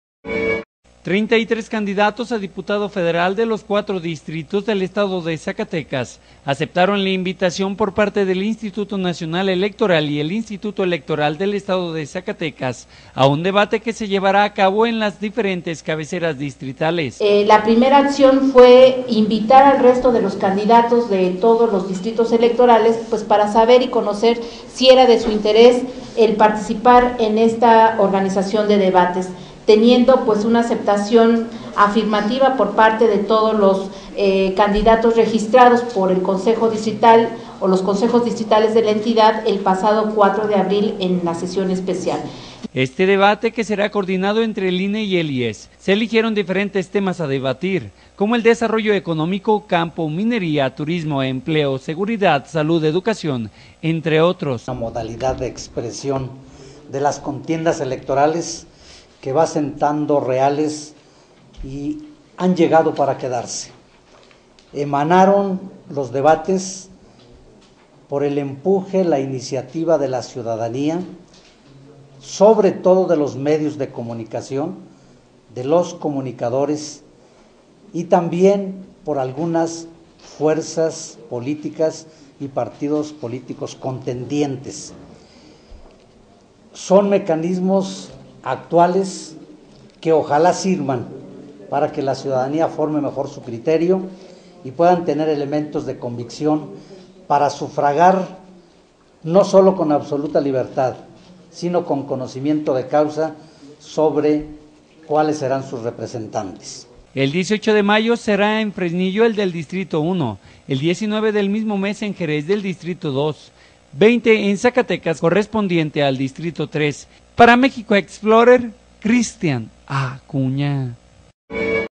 Aceptan 33 candidatos diputado federal en Zacatecas invitación al debate
33 candidatos a diputado federal de los 4 distritos del estado de Zacatecas, aceptaron la invitación por parte del Instituto Nacional electoral y el instituto electoral del estado de Zacatecas, a un debate que se llevaran a cabo en las cabeceras distritales